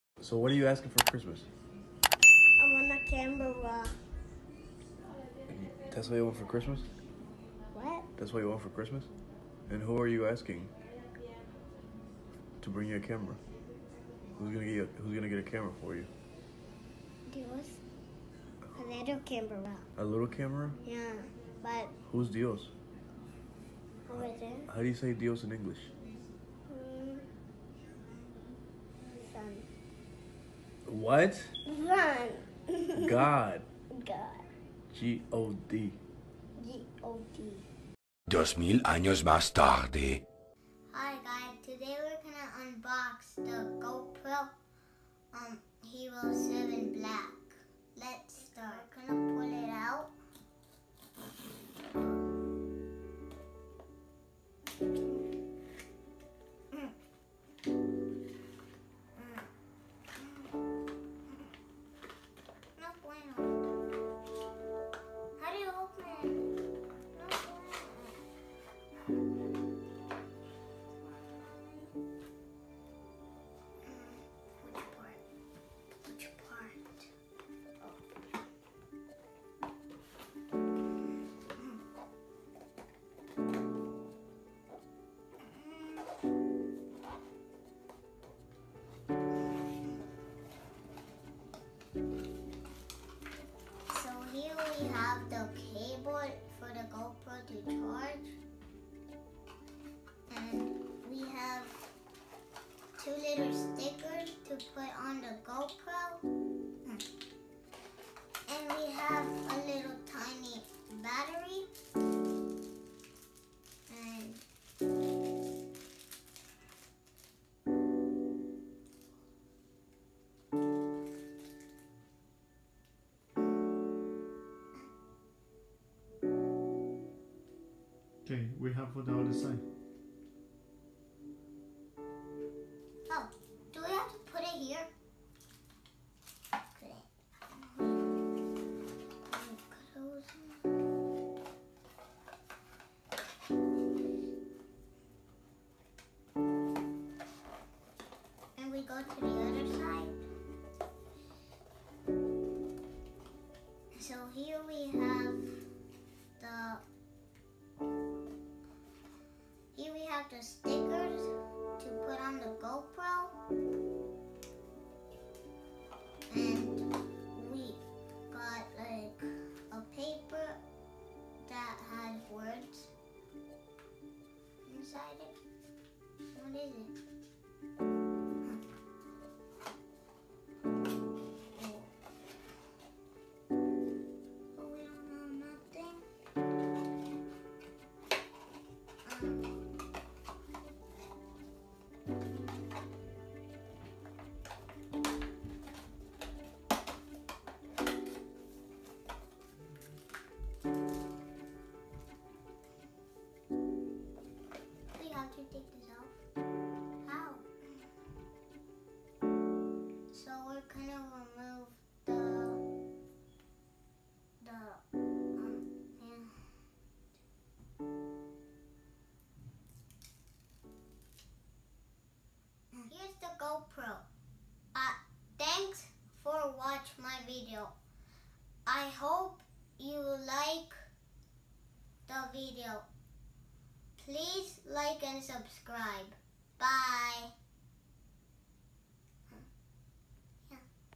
📷GOPRO HERO 7BLACK UNBOXING
👶my son with his first video on youtube was super excited,
when he opened the GoPro 7 black...👶

                    ⬇️product link ⬇️

GoPro Hero 7 Black